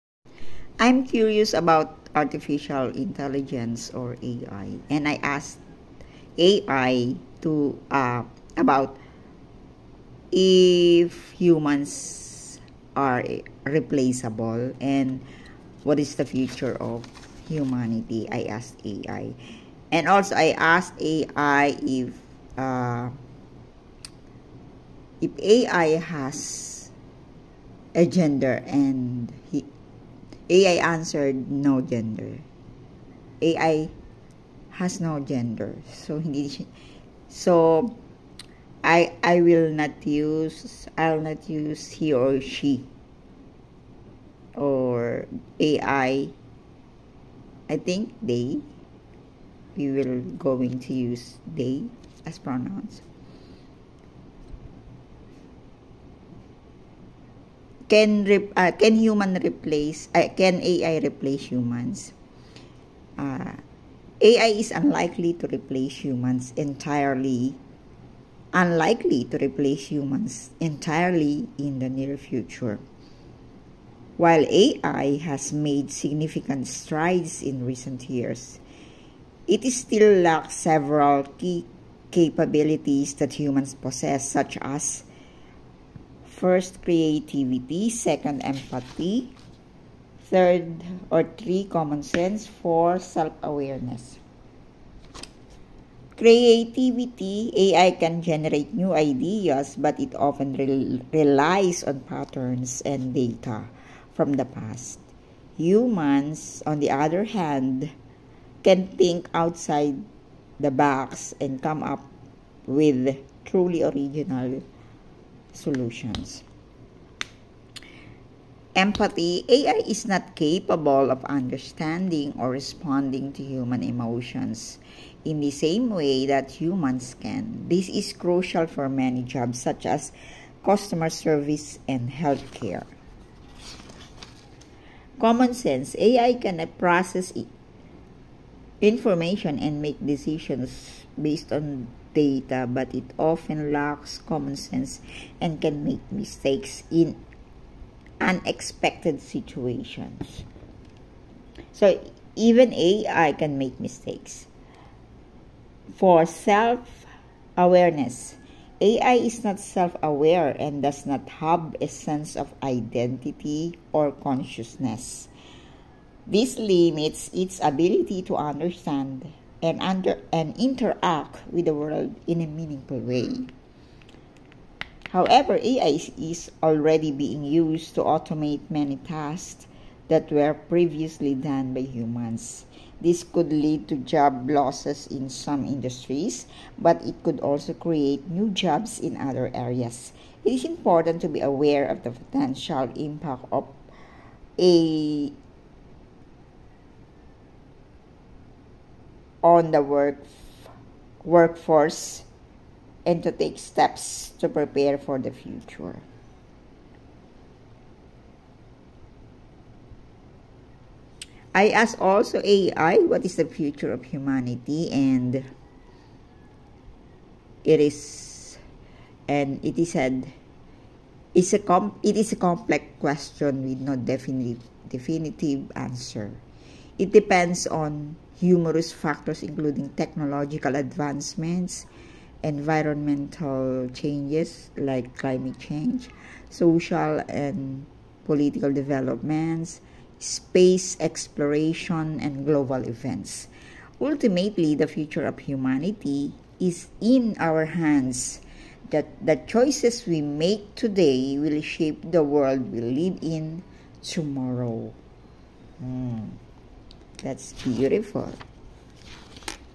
Is AI can replace humans?What is the future of humanity? I wanna share what AI's response.
On Artificial Intelligence